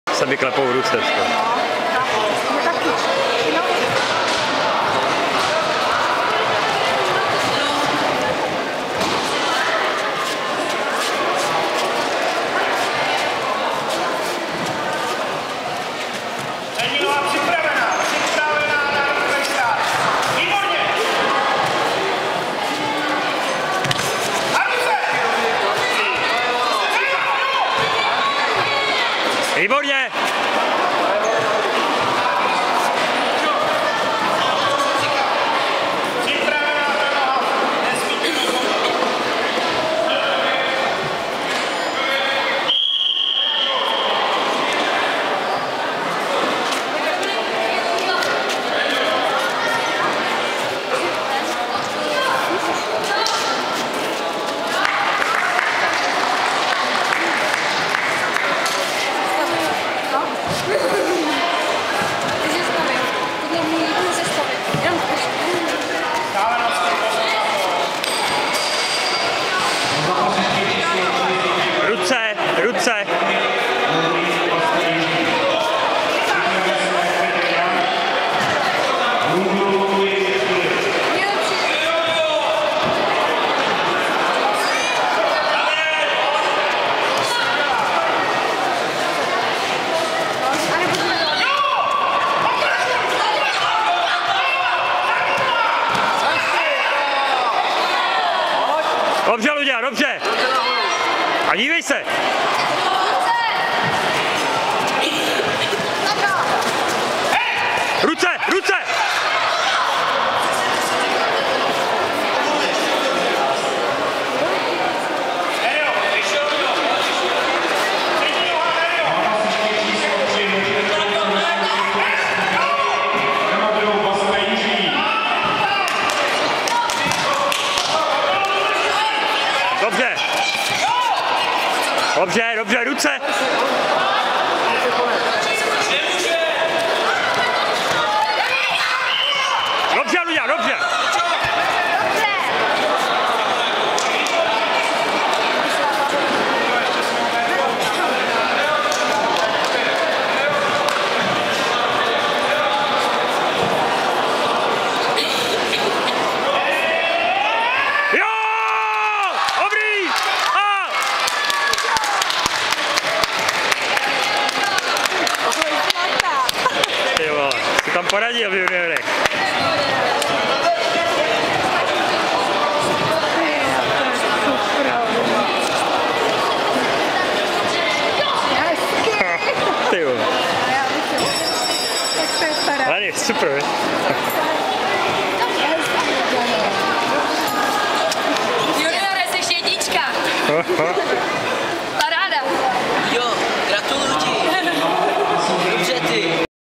Luďa (Ludvík Cigna) - Taekwon-Do STM závody - matsogi finále 20110508 2.zápas
Zlato je doma ;-)